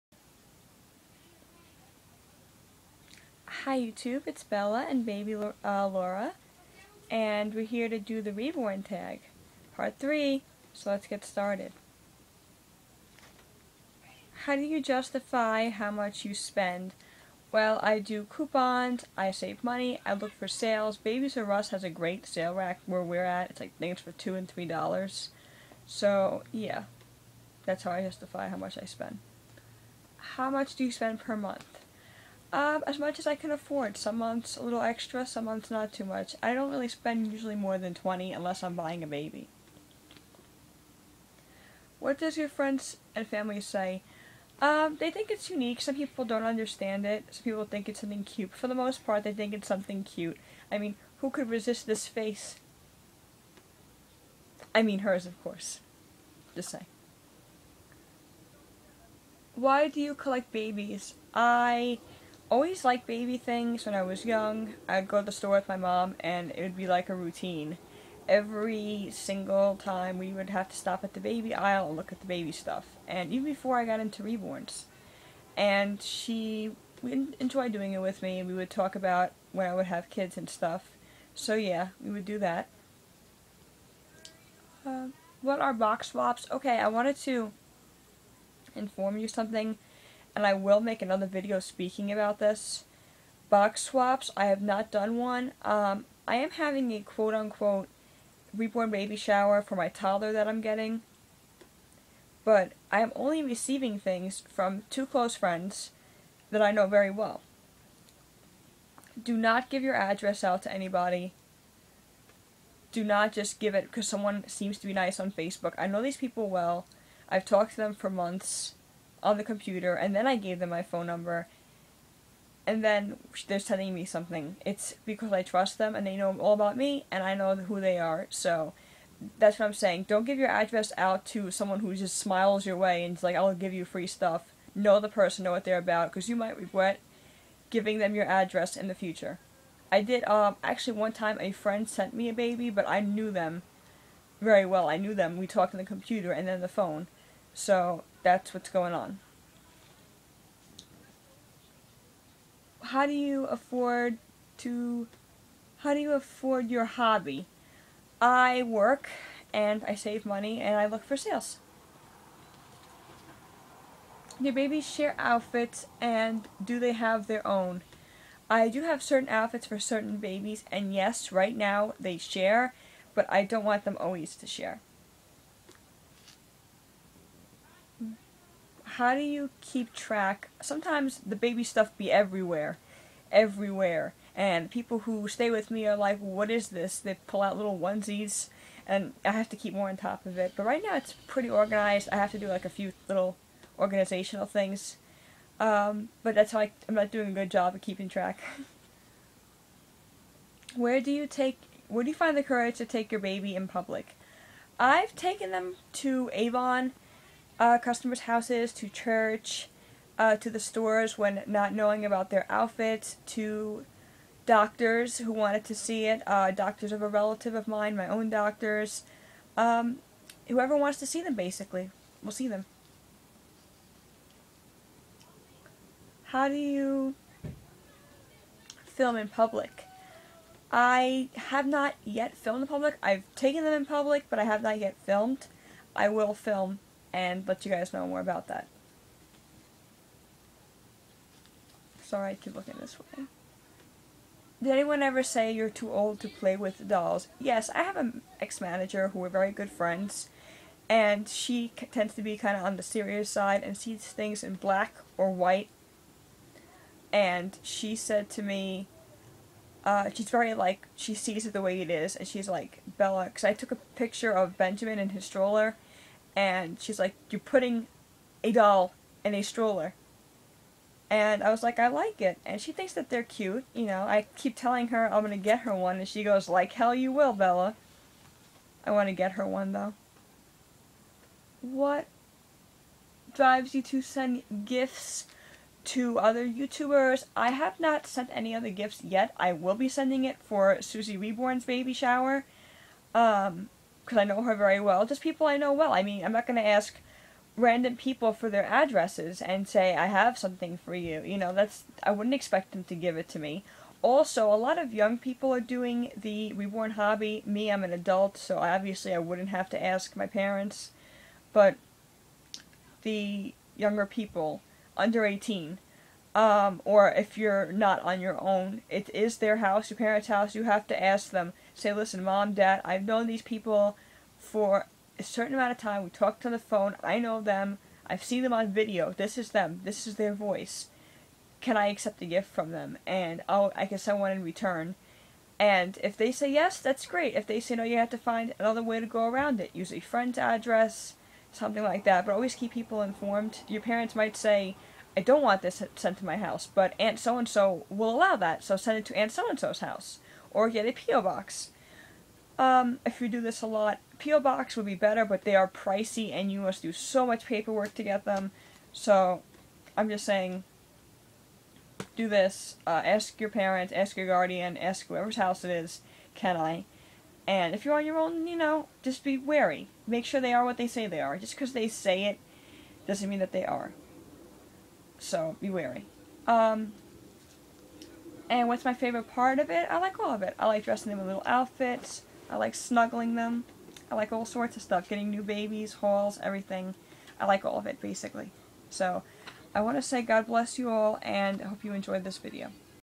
Be careful in box swaps,spending tips,the reborn tag 3
My the reborn tag 3, spending tips and how to be careful in a box swap.